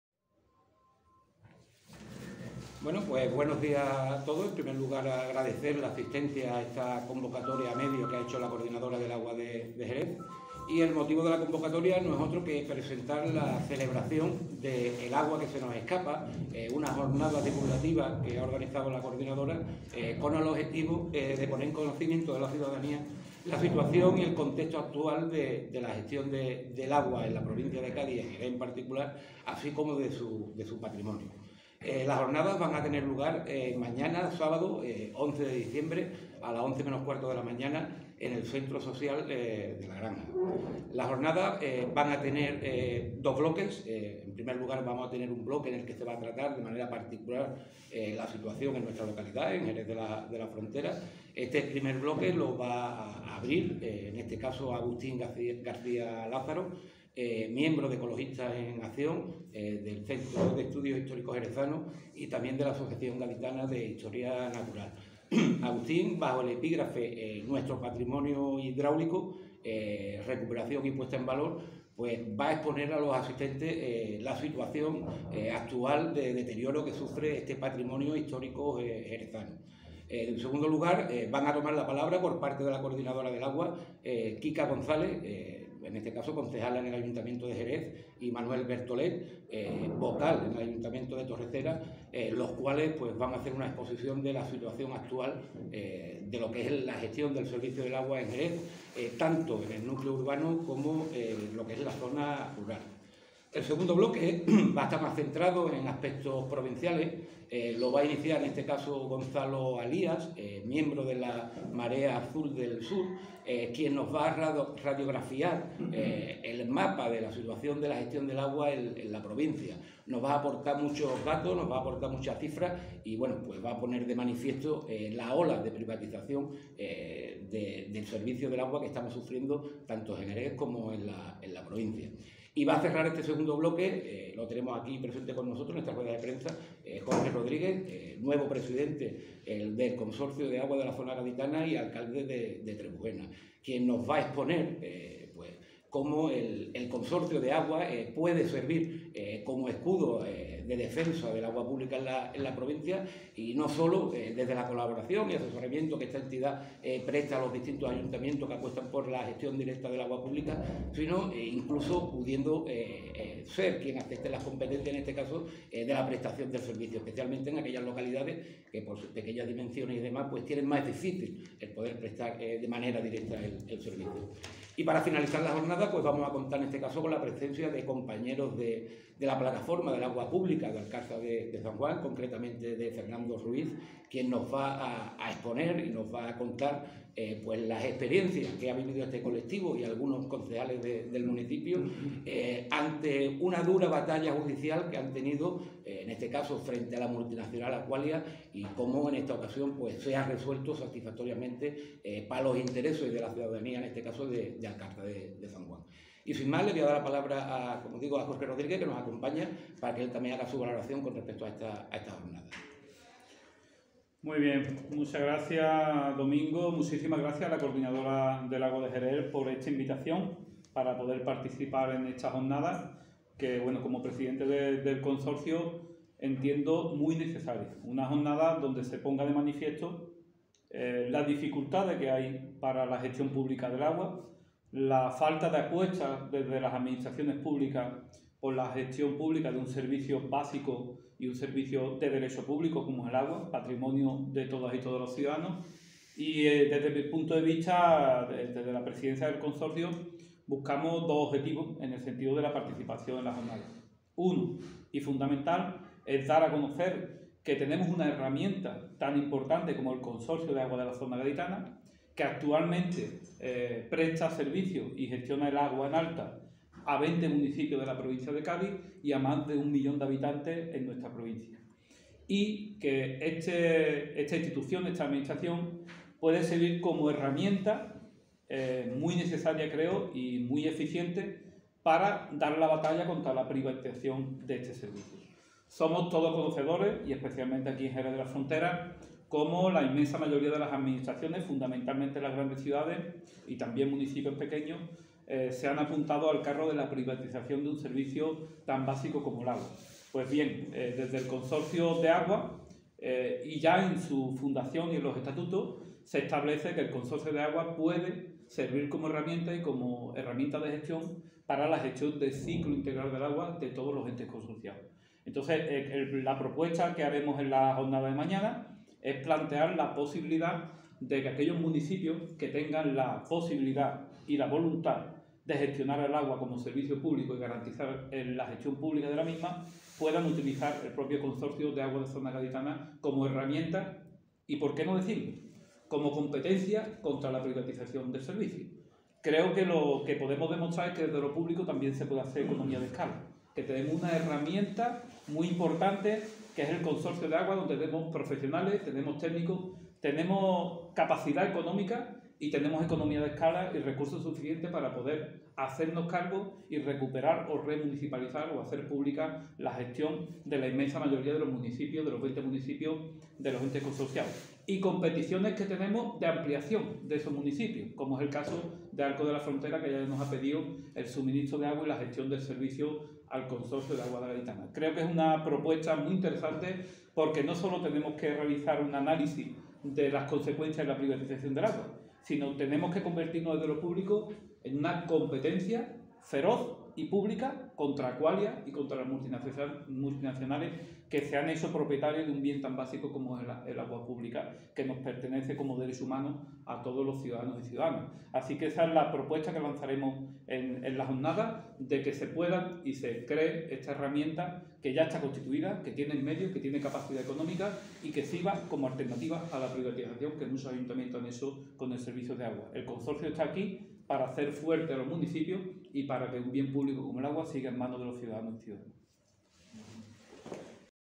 Coordinadora del Agua de Jerez celebra una jornada divulgativa sobre el patrimonio hidráulico
La Coordinadora del Agua de Jerez ha presentado la celebración de “El Agua que se nos Escapa”, nombre que reciben las jornadas divulgativas que tendrán lugar mañana, 11 de diciembre, en el Centro Social La Granja de Jerez. Domingo Pedro García, miembro de la Comisión Permanente de la Coordinadora del Agua, ha desgranado los detalles de las jornadas que abogan por poner en valor el patrimonio hidráulico y la gestión pública del agua, un servicio esencial y de derecho público para todos los ciudadanos.